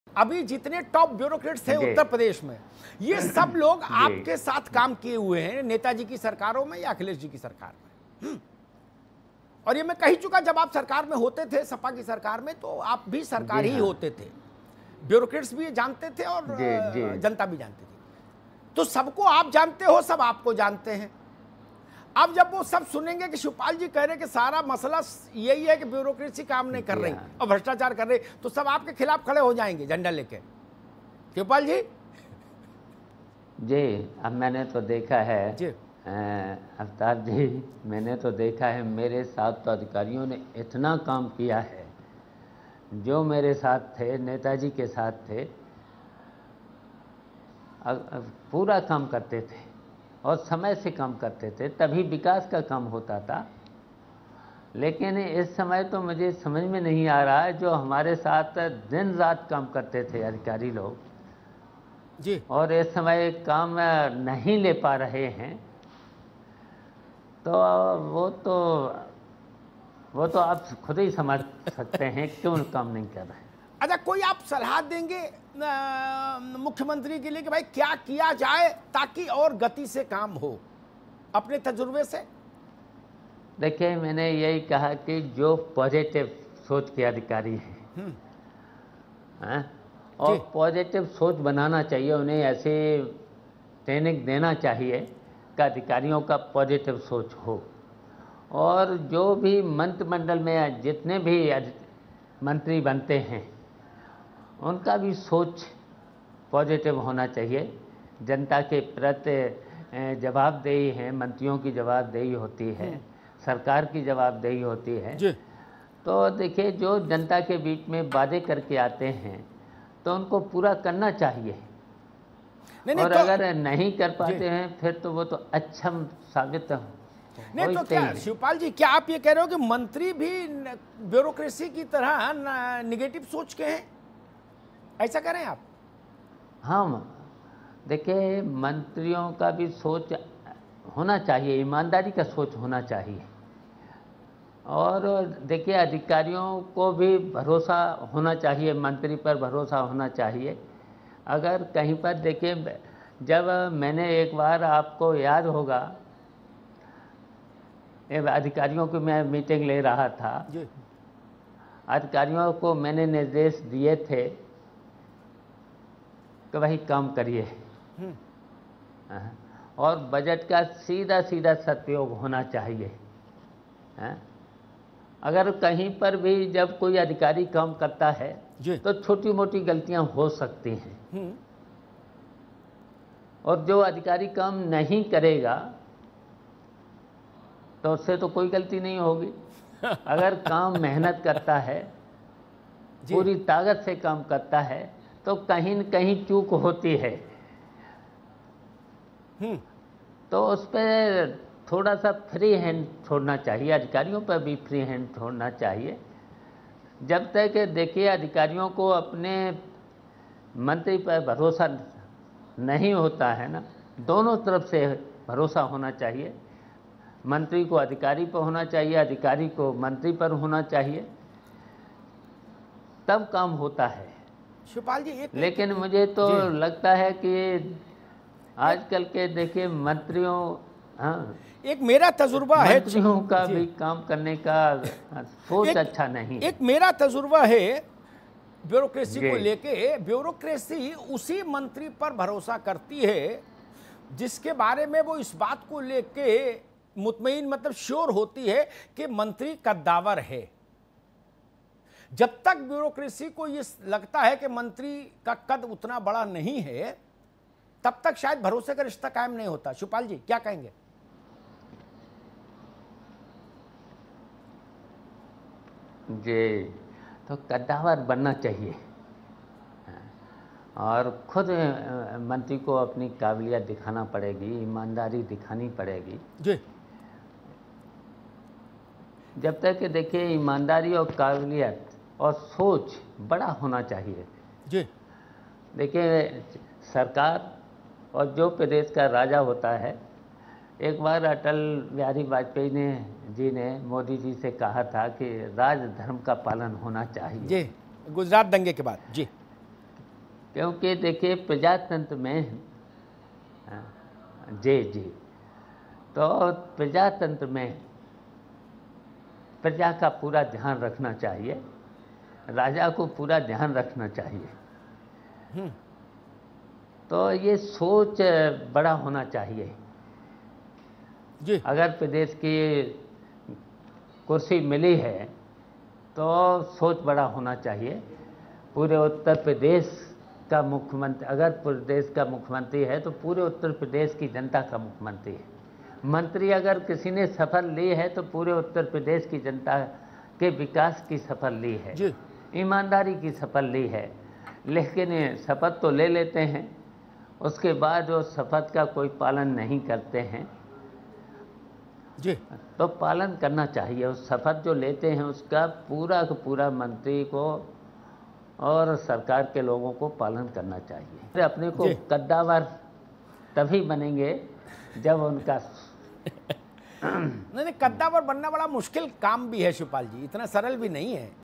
नेता जी और मेरे साथ काम करने वाले अधिकारी आज की सरकार में काम नहीं कर पा रहे हैं- शिवपाल यादव R9TV
R9 TV पर बोले शिवपाल यादव
सभी समाजवादी लोग एक मंच पर रहें -शिवपाल यादव
हम सभी नेता जी को नेता मानते हैं -शिवपाल यादव
'गठबंधन में सभी ने नेता जी को नेता स्वीकार कर लिया था'
एक बयान ने सभी काम को बिगाड़ दिया -शिवपाल यादव